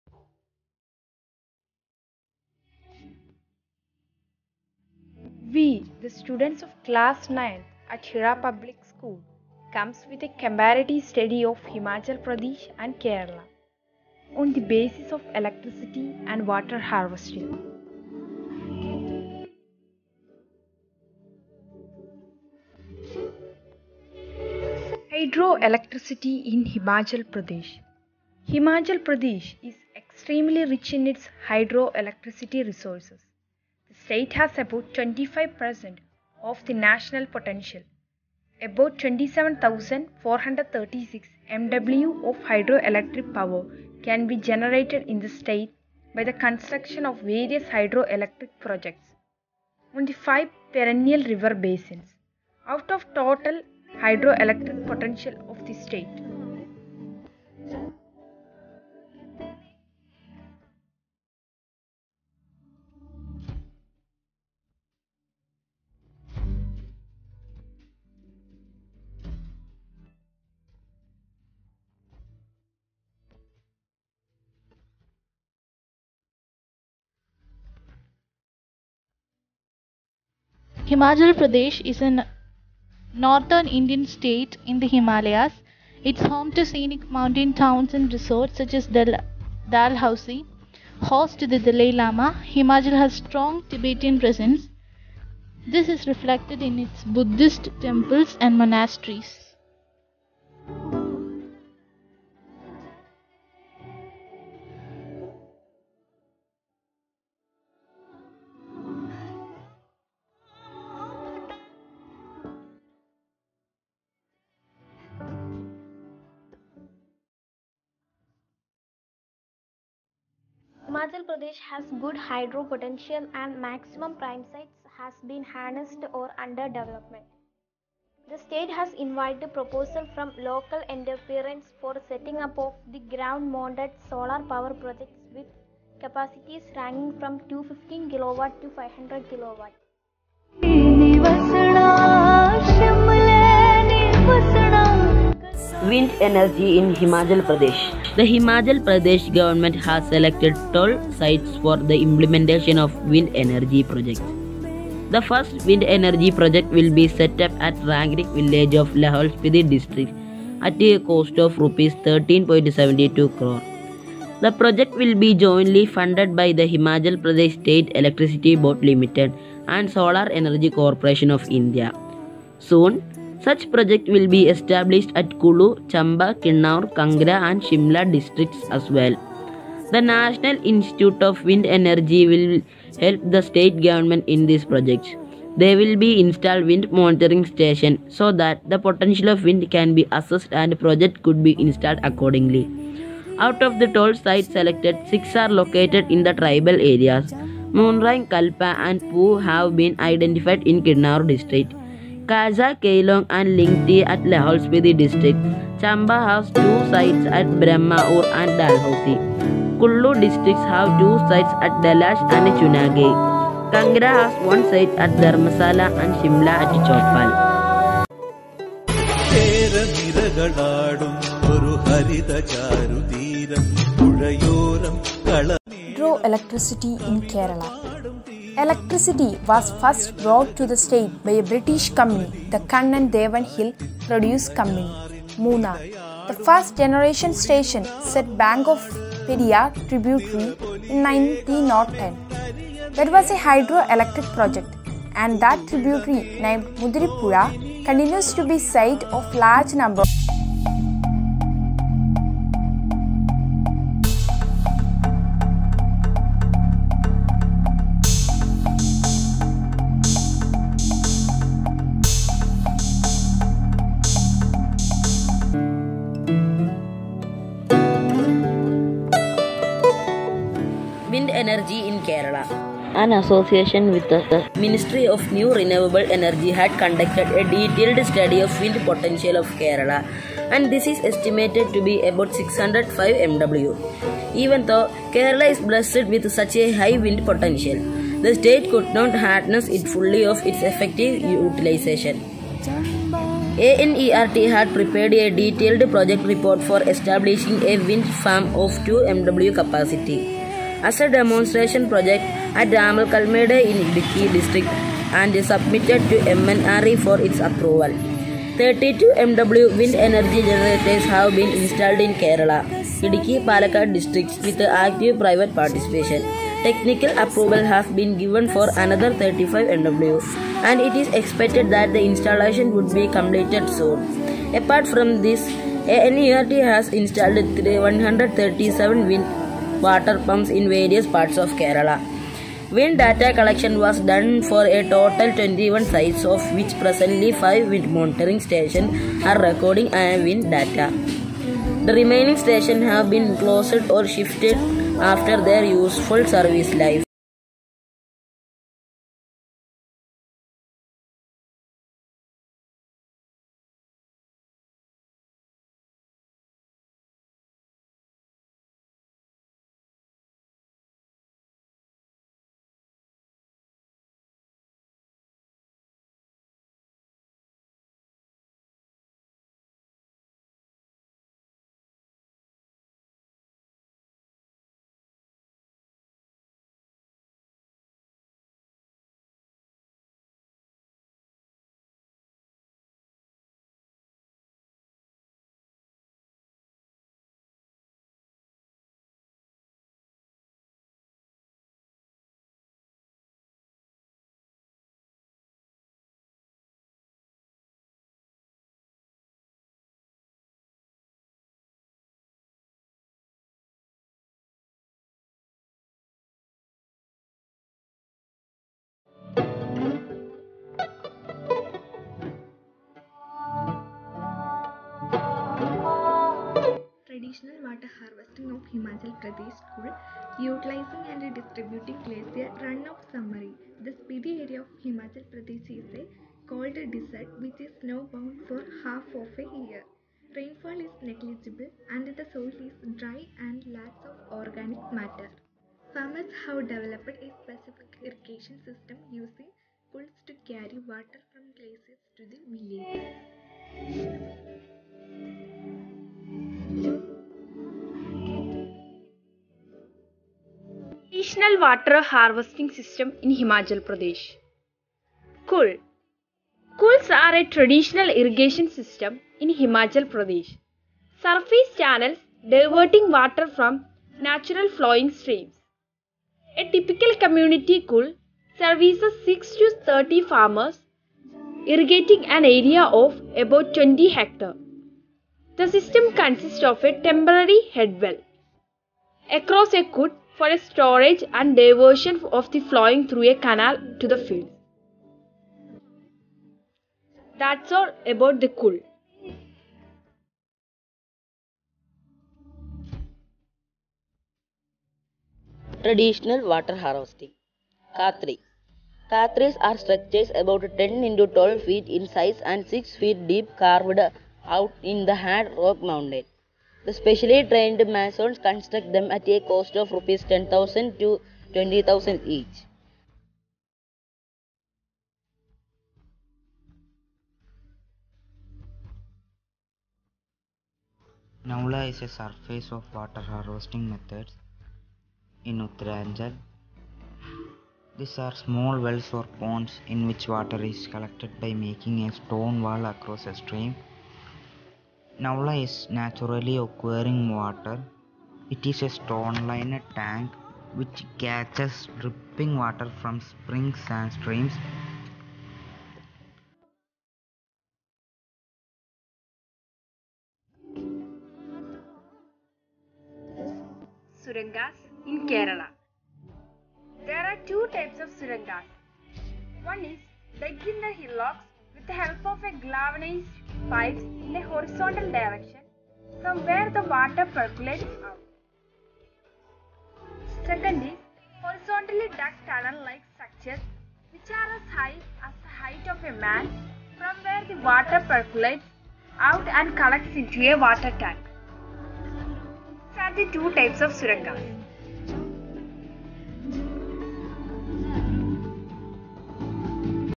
Kerala and Himachal Pradesh |Art integrated project |Science|Electricity
As a part of the  implementation of Art Integrated Learning project students of grade 9 has taken a huge  effort to make a comparative study of Kerala and Himachal Pradesh.